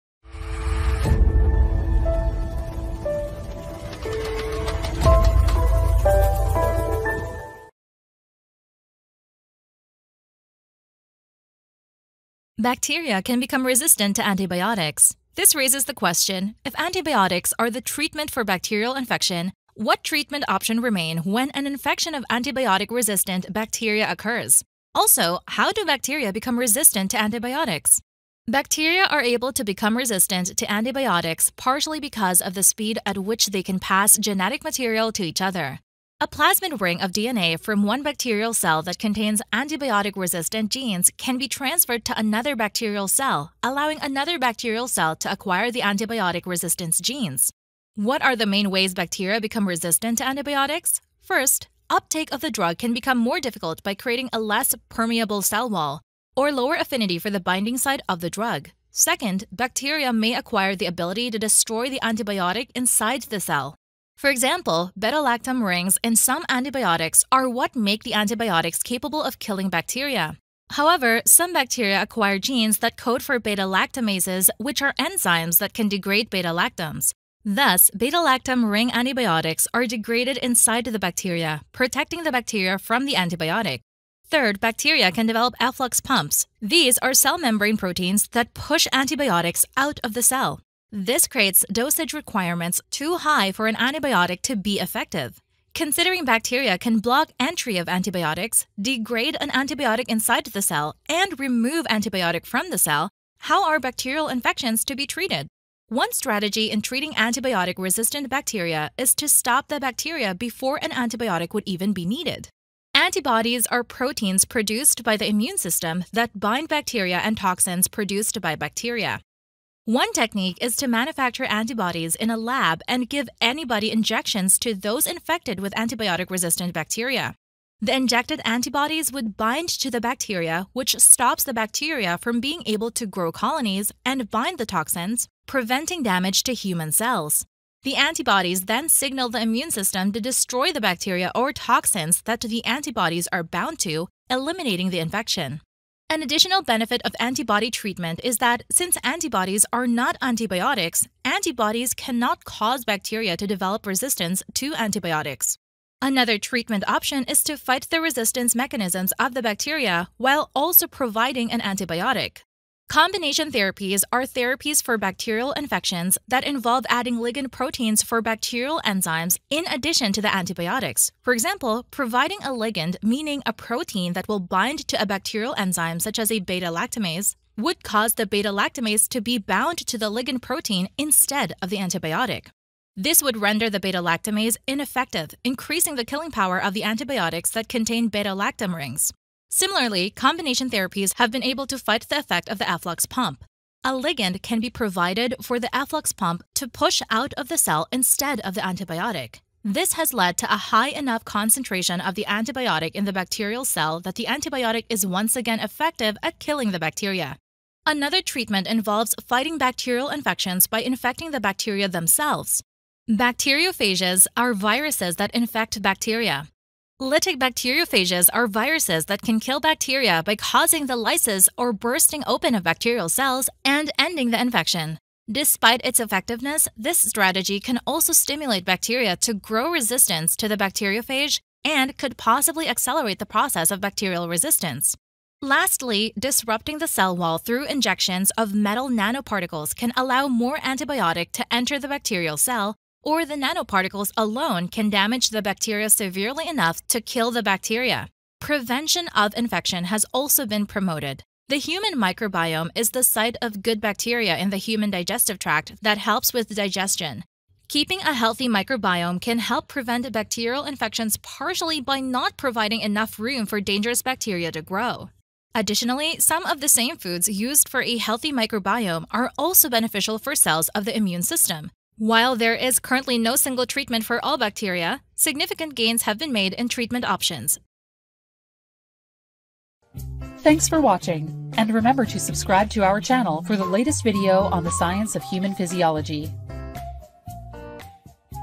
ANTIBIOTIC RESISTANT BACTERIA! How do they become resistant? Can they still be killed?
This video covers the topics of how antibiotics become resistant to bacteria and possible ways to treat bacteria that have become resistant to antibiotics.

Some of the journal articles used in the production of this video are as linked here:

IJMS | Free Full-Text | Defeating Antibiotic-Resistant Bacteria: Exploring Alternative Therapies for a Post-Antibiotic Era

Pathogens | Free Full-Text | Bacterial Antibiotic Resistance: The Most Critical Pathogens

Renaissance of vancomycin: approaches for breaking antibiotic resistance in multidrug-resistant bacteria